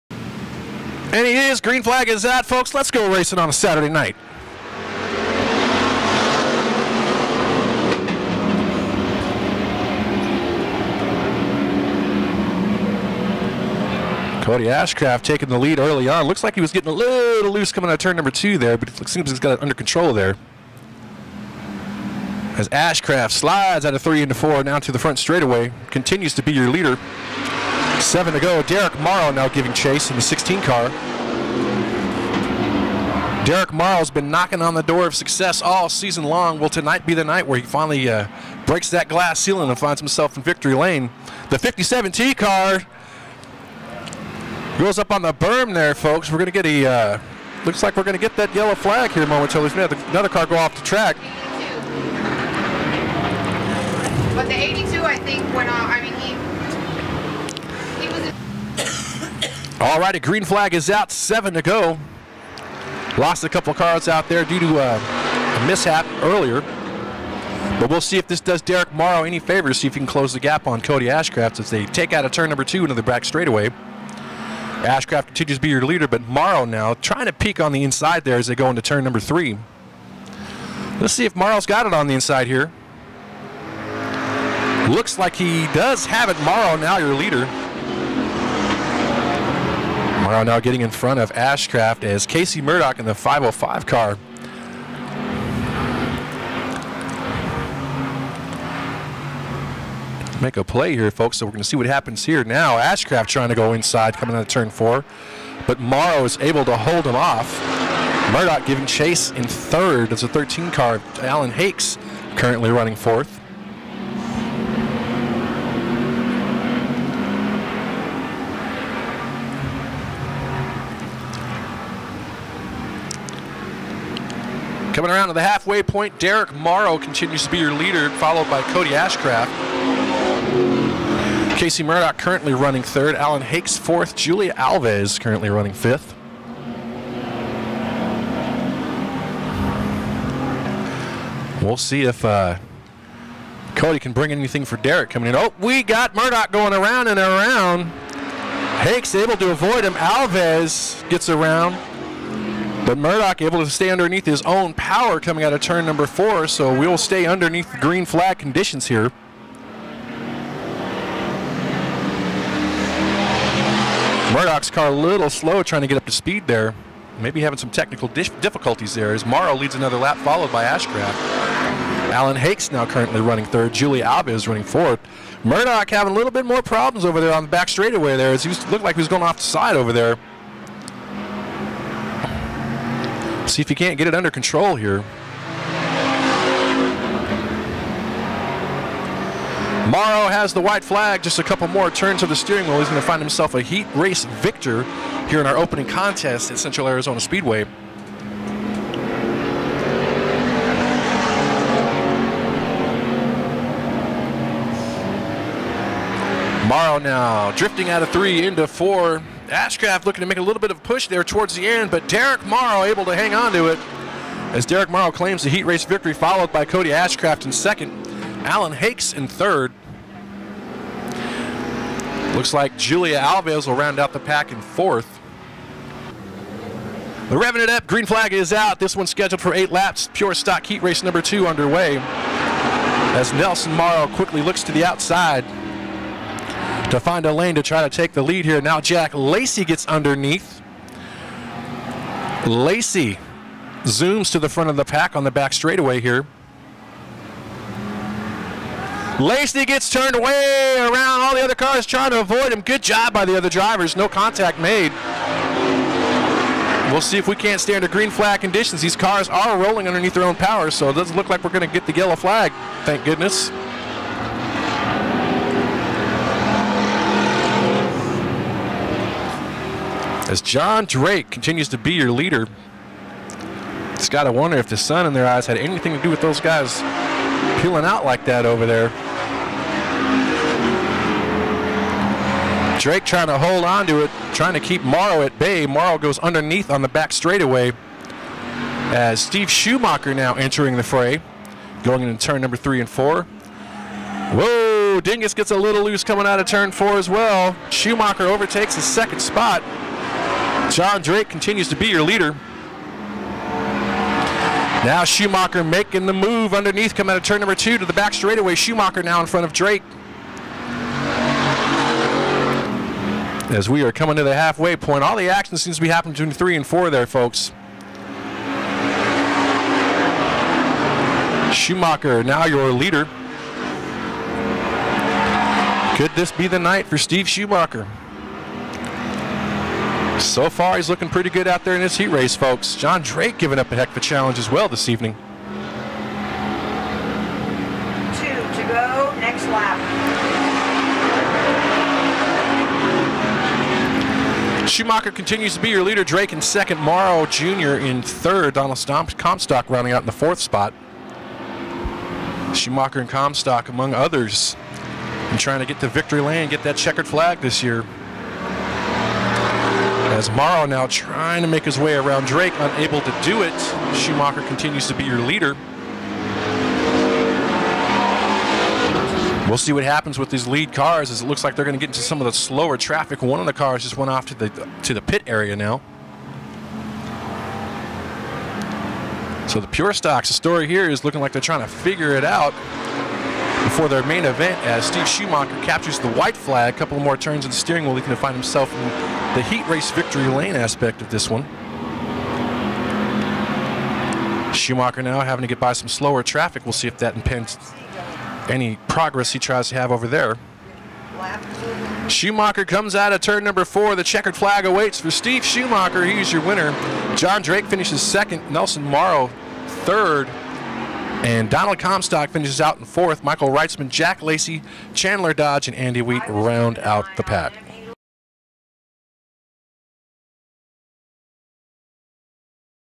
Central Arizona Speedway Pure Stock Heats June 22 2019
Gestalt Production Presents the Pure Stock Heats from Central Arizona Speedway on June 22, 2019!